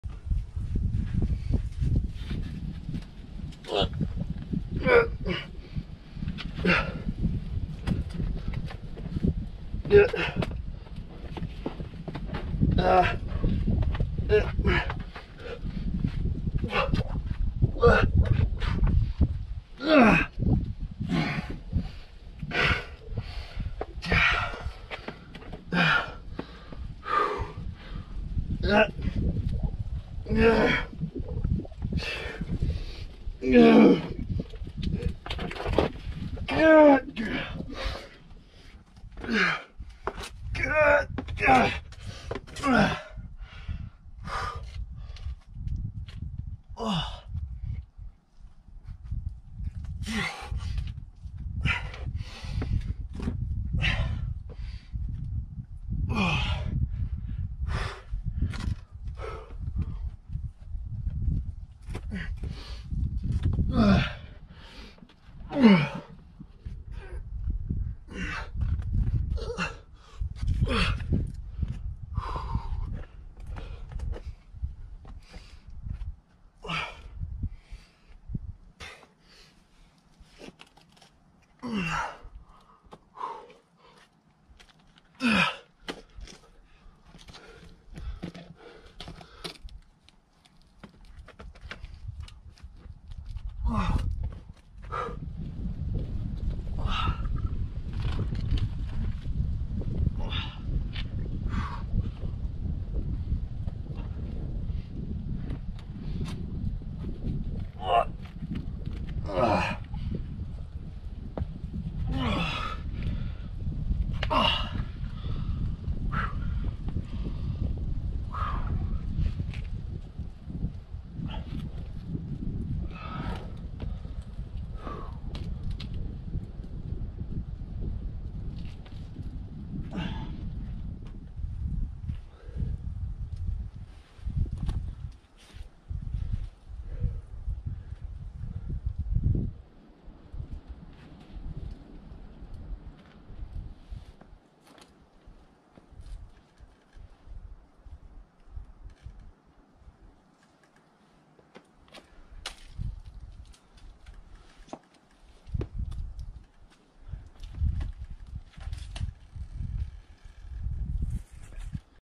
Valley Falls Bouldering: The Architect, V7 First Ascent
First ascent of The Architect, V7 at the main area at Valley Falls State Park in Fairmont, WV.